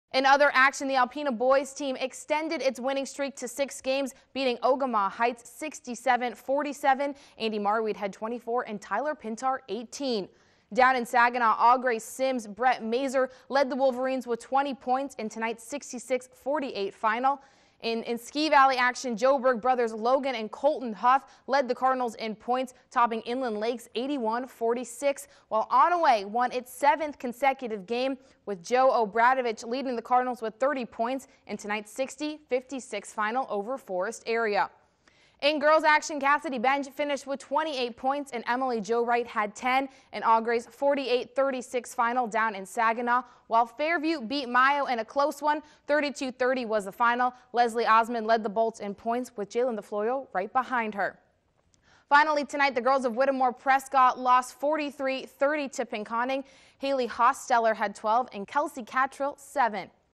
January 21, 2014: High School Basketball Scores
Boys and girls scores from around the area.